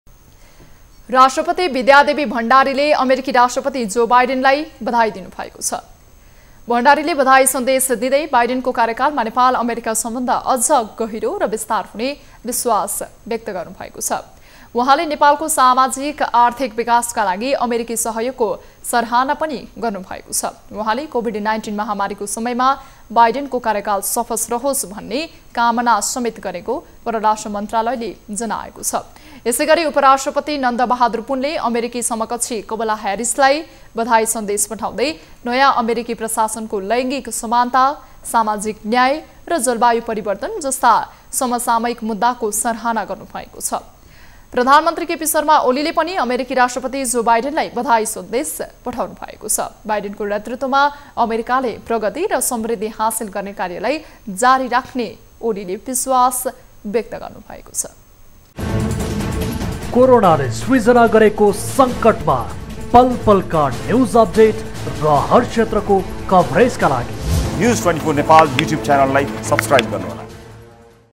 राष्ट्रपति भण्डारी र प्रधानमन्त्री ओलीले दिए बाइडनलाई बधाई - NEWS24 TV
राष्ट्रपति विद्यादेवी भण्डारीले अमेरिकी राष्ट्रपति जो बाइडनलाई बधाई दिनुभएको छ ।  भण्डारीले बधाई सन्देश दिँदै बाइडनको कार्यकालमा नेपाल अमेरिका सम्बन्ध अझ गहिरो र विस्तार हुने विश्वास व्यक्त गर्नुभएको छ । उहाँले नेपालको सामाजिक आर्थिक विकासका लागि अमेरिकी सहयोगको सह्राना पनि गर्नुभएको छ । उहाँले कोभिड–१९ महामारीको समयमा बाइडनको कार्यकाल सफल रहोस् भन्ने कामनासमेत गरेको परराष्ट्र मन्त्रालयले जनाएको छ । यसैगरी उपराष्ट्रपति नन्दबहादुर पुनले अमेरिकी समकक्षी कमला ह्यारिसलाई बधाई सन्देश पठाउँदै नयाँ अमेरिकी प्रशासनको लैंगिक समानता, सामाजिक न्याय र जलवायु परिवर्तन जस्ता सामयिक मुद्दाको सह्राना गर्नुभएको छ । प्रधानमन्त्री केपी शर्मा ओलीले पनि अमेरिकी राष्ट्रपति जो बाइडनलाई बधाई सन्देश पठाउँनु भउको छ । बाइडनको नेतृत्वमा अमेरिकाले प्रगति र समृद्धि हासिल गर्ने कार्यलाई जारी राख्ने ओलीले विश्वास व्यक्त गर्नुभएको छ ।